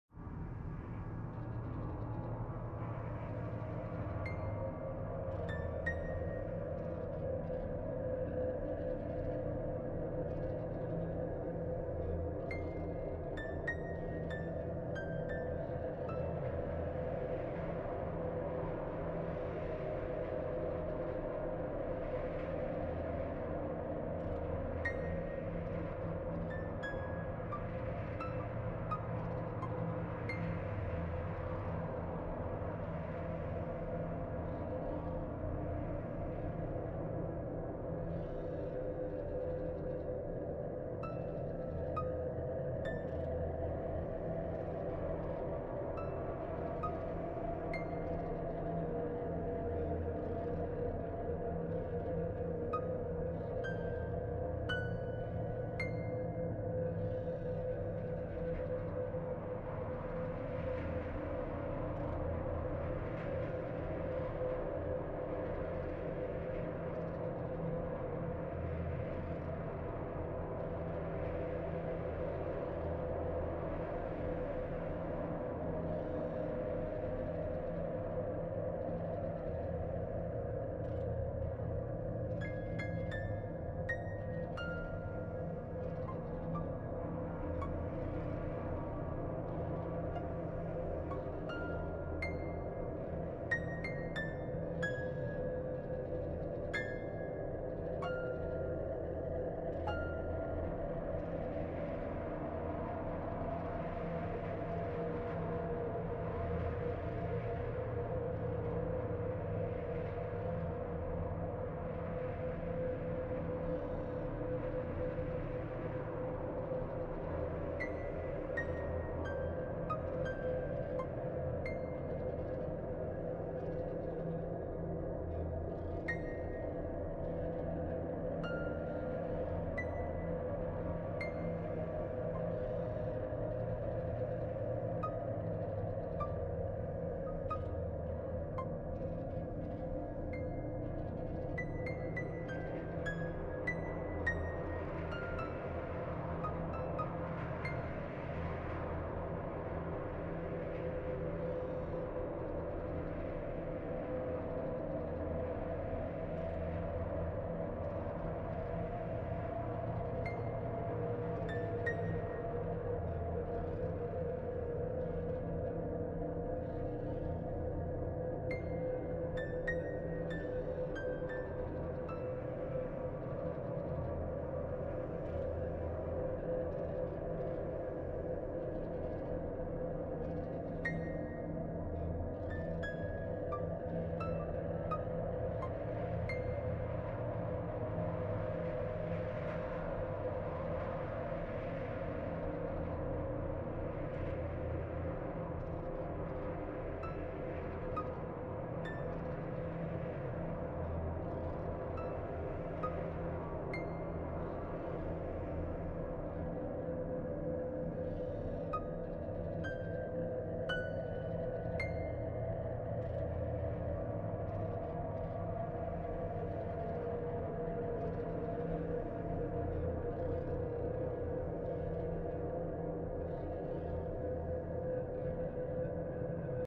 Kempton Park Hospital (South Africa)
Some Photos I took at the Abandoned Kempton Park Hospital in South Africa